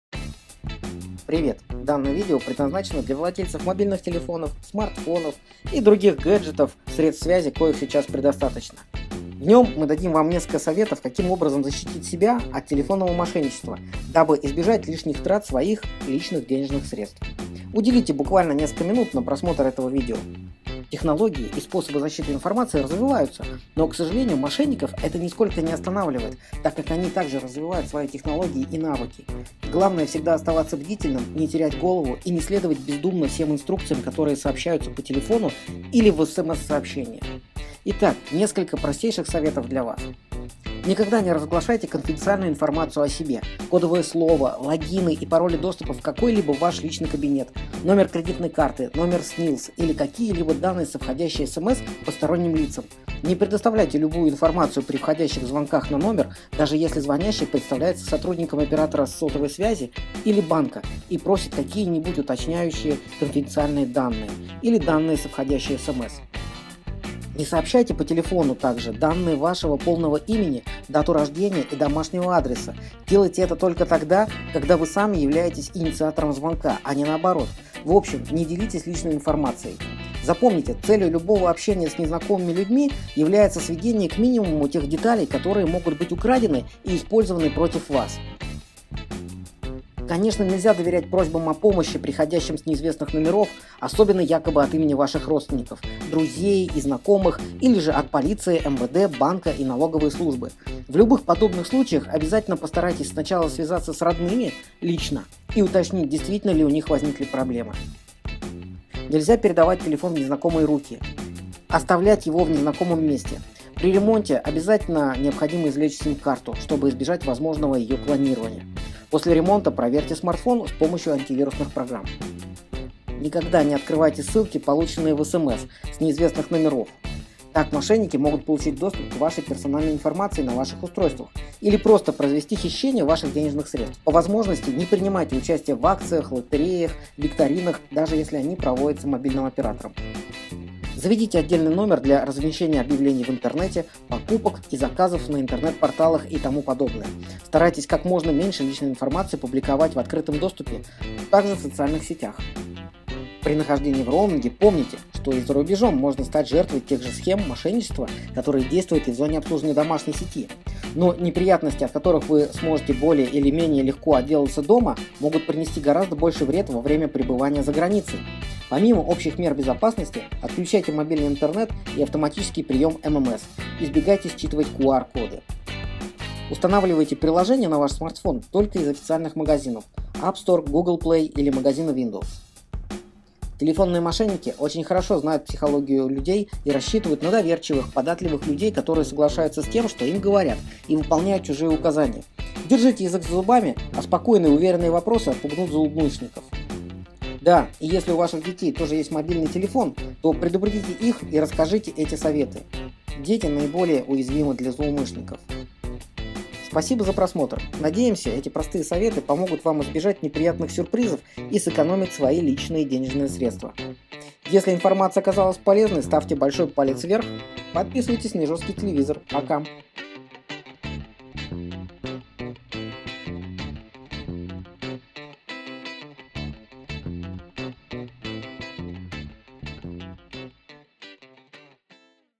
Как защититься от телефонных мошенников? (Полезная информация)
Технологии и способы защиты информации развиваются, но к сожалению, мошенников это нисколько не останавливает так как они также развивают свои технологии и навыки. В этом видео мы дадим Вам несколько советов каким образом защитить себя от телефонного мошенничества, дабы избежать потерь своих денежных средств. Уделите буквально несколько минут на просмотр этого видео.
Но, всё-таки, главное всегда оставаться бдительным, не терять голову и не следовать бездумно всем инструкциям, которые сообщаются по телефону или в СМС.

Наш короткий адрес: izhoratv.ru

И немножко хэштегов